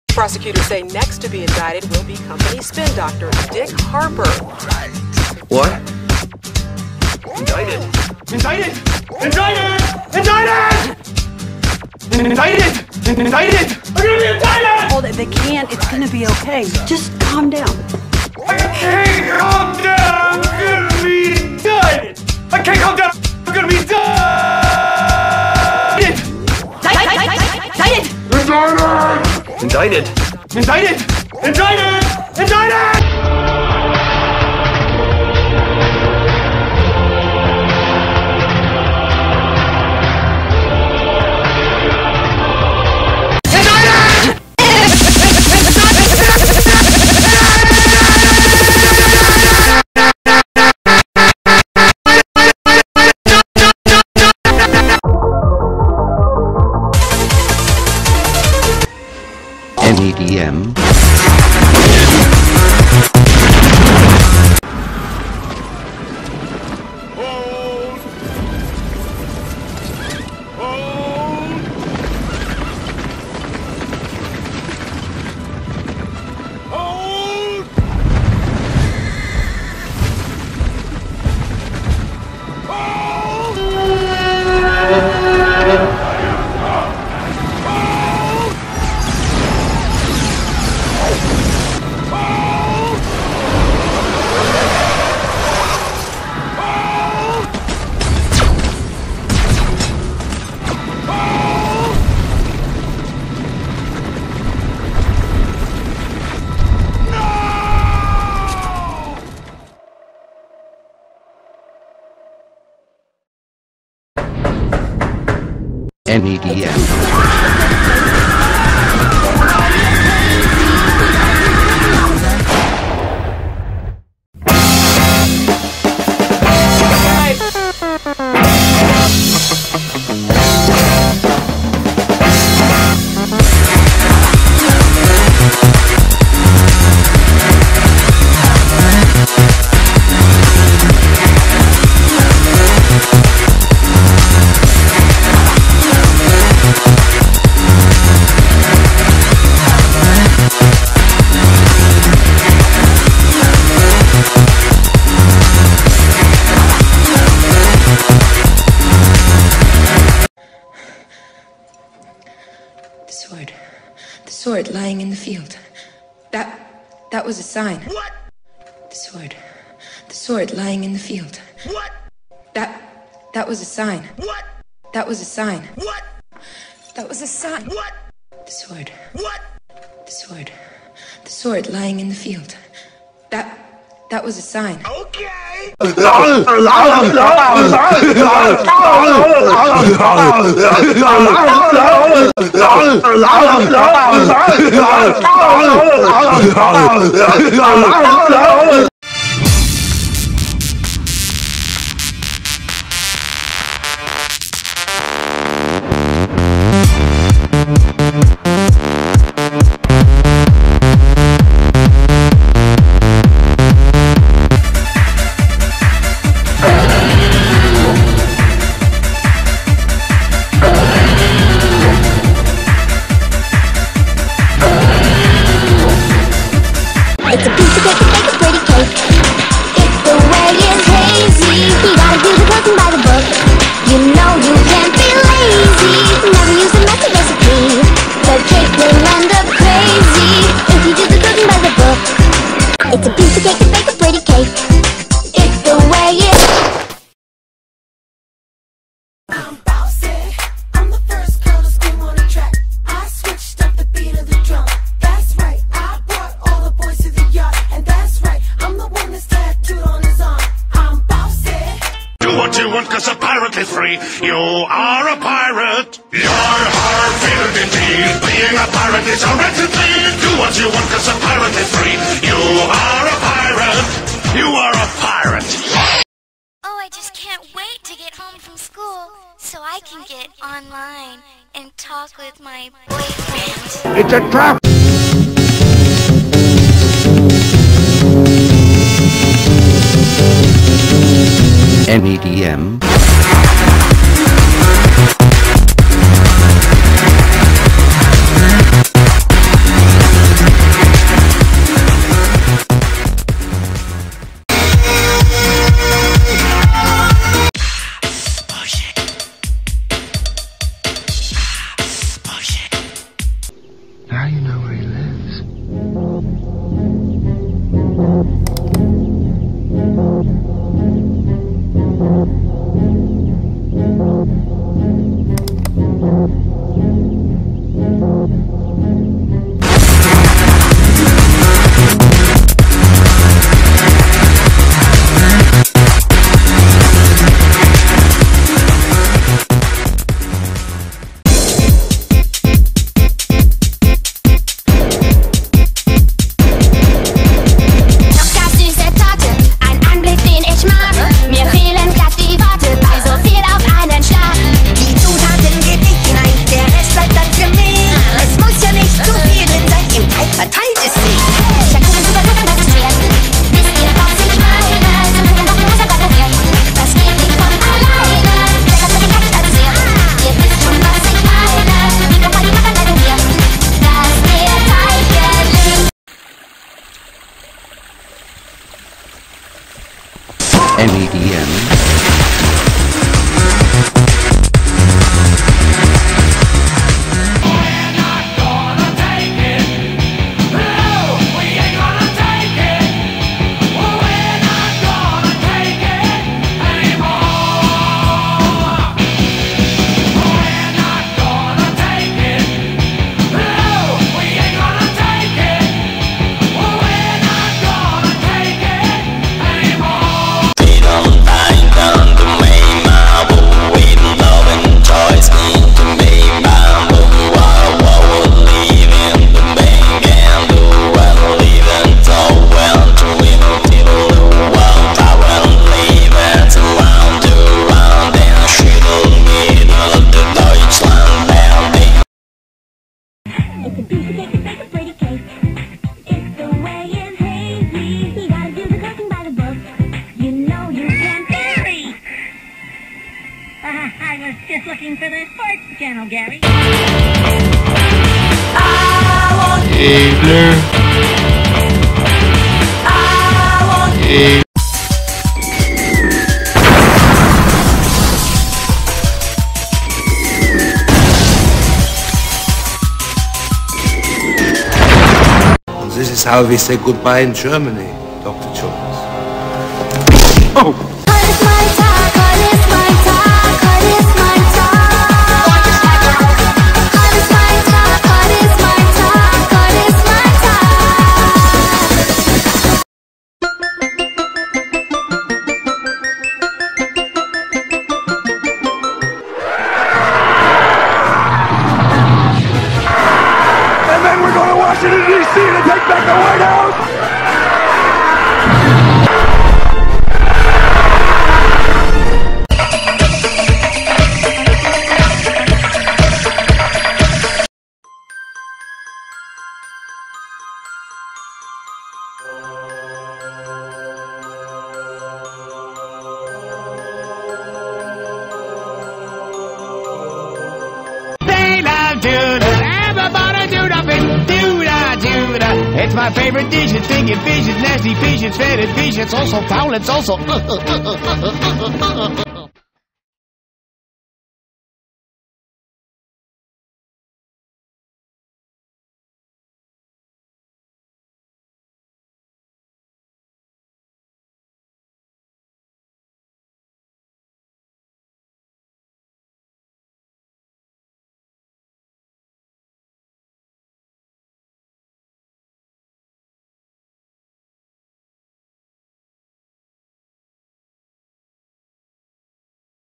My YTMND Gifs Compilation
This is my Gif compilation of YTMND's, in HD720p
Some of this YTMND's you may had seen them before and they may be pretty old, but you'll find some new talent too!.

Please consider this:
Some YTMND's are made very small size and they have been zoom'd so they maybe look a little outsized and pixel'd,
but it will be even more/better quality than others' compilations

another thing that has been consider...
..List of links!!! (in order of appearance)

9.- NEDM: Revenge!:
 Link Dead

11.- The sword... WHAT?!:
 Link Dead

22.- ? HERMIONE AND CONAN GET REAL FREAKY, REMIX WITH ASIAN KID PART II:

  "NEDM - What will happen if you burn cats"

41.- exsterenpro's YouTube Avatar

* = No longer available... Changed

(YTMNDs' names will come later)